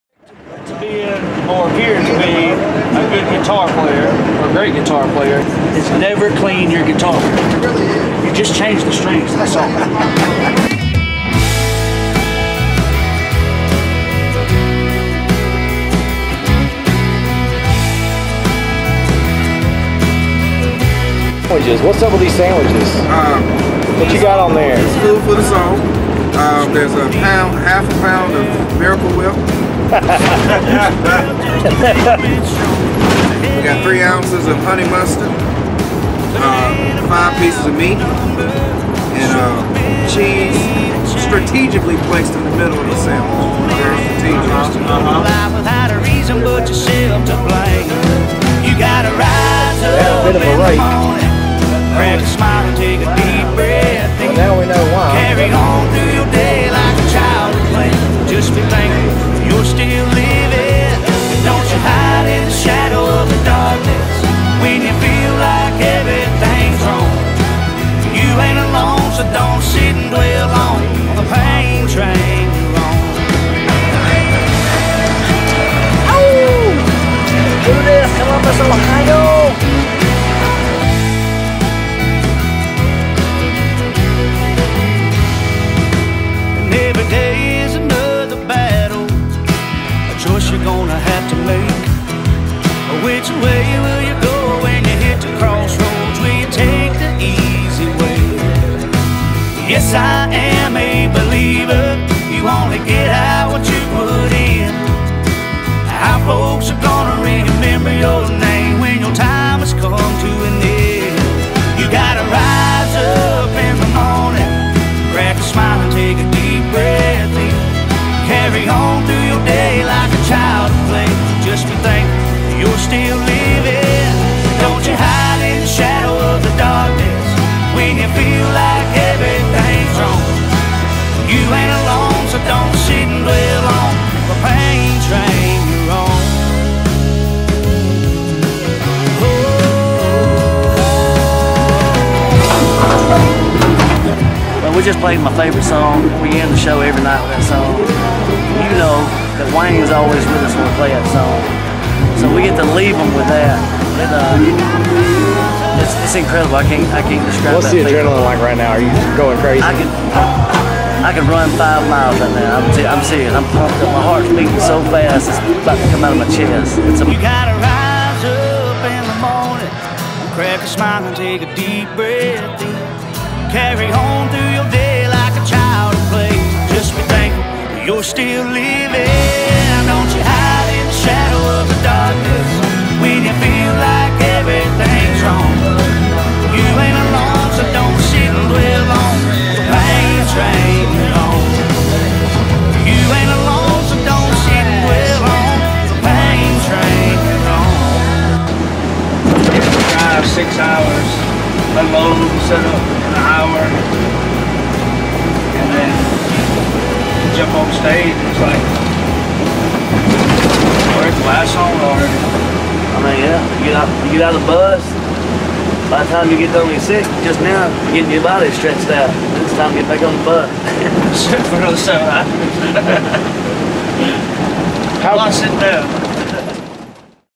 On the Road with WBB
Follow along which the Wilson Brothers Band on the Budweiser tour